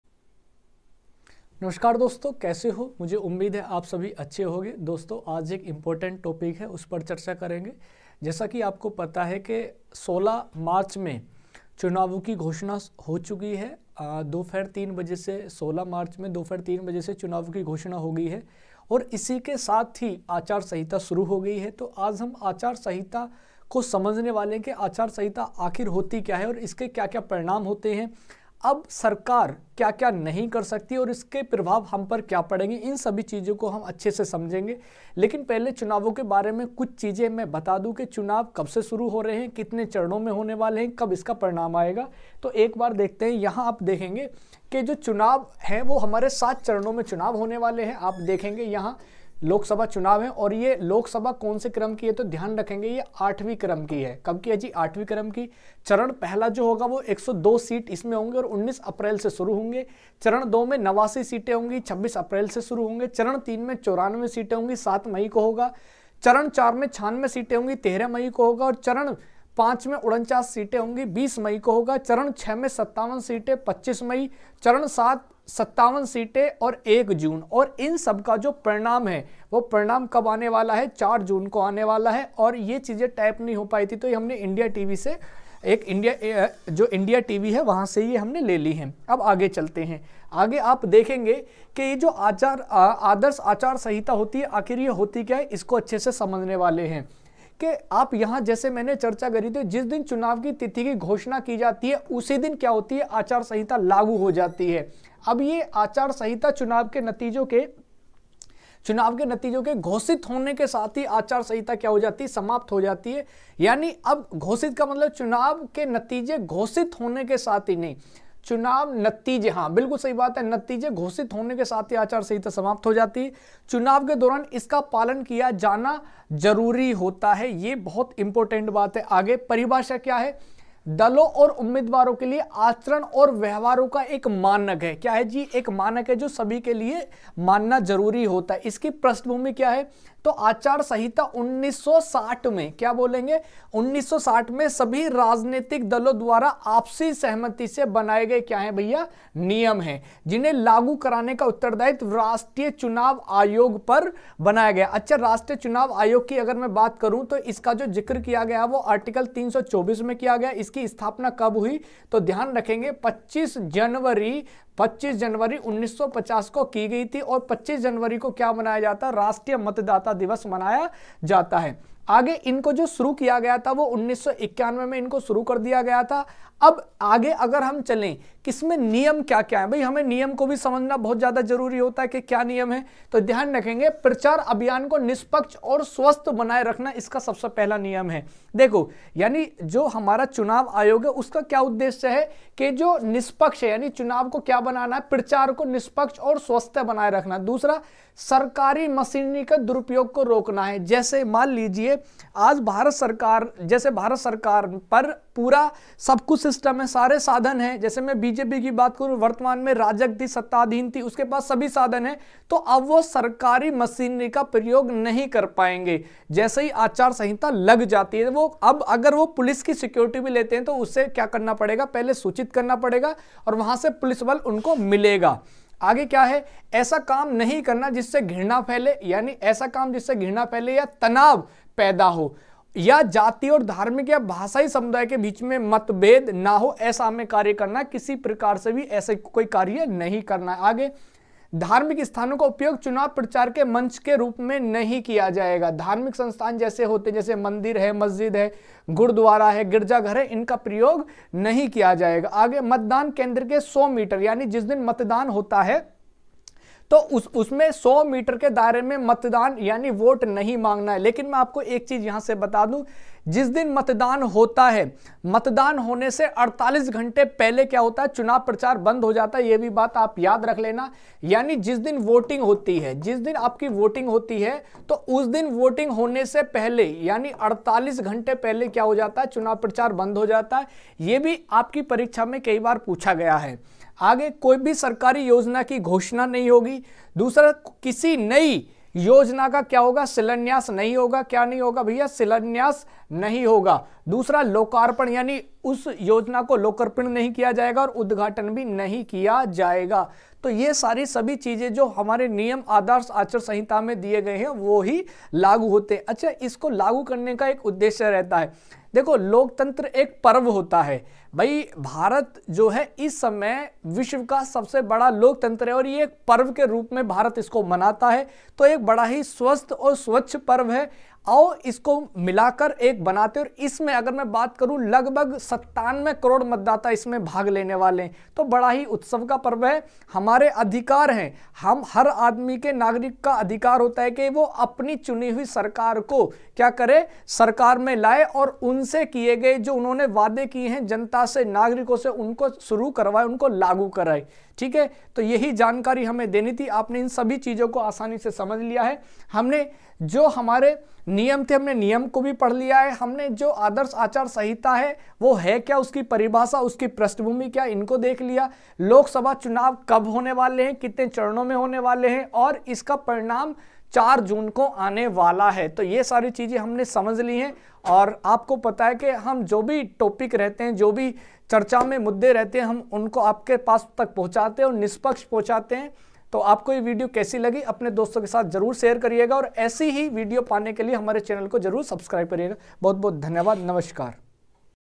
चुनावों की घोषणा 2024। आचार संहिता शुरु ✅ 18th लोक सभा चुनाव! क्या- क्या हों सकता है आचार संहिता ?
2023 ke chunav ki ghoshna